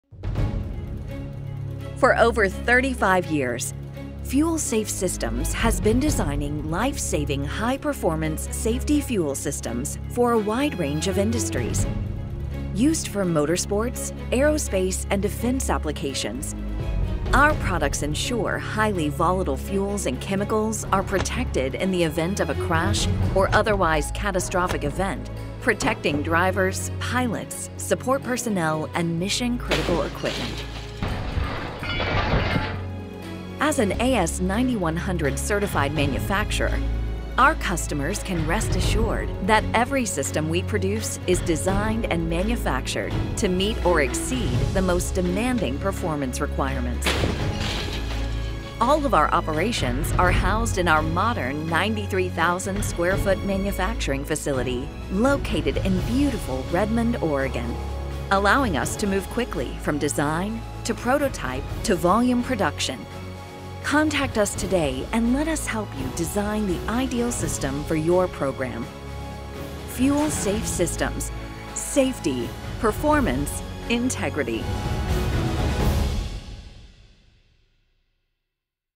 Our Story - Fuel Safe Systems
For over 35 years, Fuel Safe Systems has been designing life saving high-performance safety fuel systems for a wide range of industries.  Used for motorsports, aerospace, and defense applications, our products ensure highly volatile fuels and chemicals are protected in the event of a crash or otherwise catastrophic event protecting drivers, pilots, support personnel and mission critical equipment.

As an AS9100-certified manufacturer, our customers can rest assured that every system we produce is designed and manufactured to meet or exceed the most demanding performance requirements.

All of our operations are housed in our modern, 93,000 sq. ft. manufacturing facility, located in beautiful Redmond, Oregon allowing us to move quickly from design, to prototype, to volume production.

Fuel Safe Systems - Safety, Performance, Integrity.